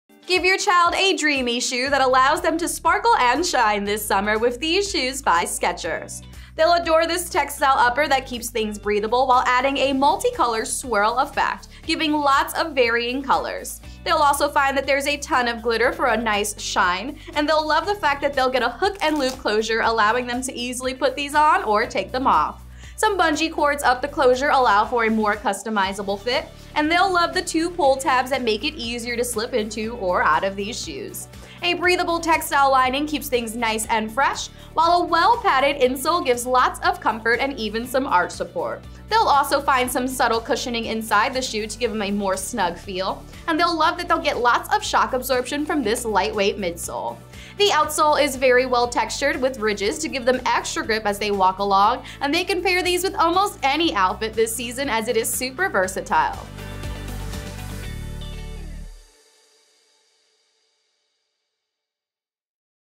SKECHERS KIDS Sport - Dreamy Lites 303512L (Little Kid/Big Kid) SKU: 9684754
SKECHERS KIDS SIZE CHART
Shimmer and shine through the day with fun colors wearing the SKECHERS&reg; KIDS Sport - Dreamy Lites 303512L sneakers.
Multicolor swirl print fabric upper with sparkle finish.
Cushioned comfort insole.
Shock-absorbing midsole.
Hook-and-loop instep strap closure.
Lightweight slip-on style with stretch laces.
Skechers&reg; logo detail.
Flexible traction outsole.
Machine washable.
Imported.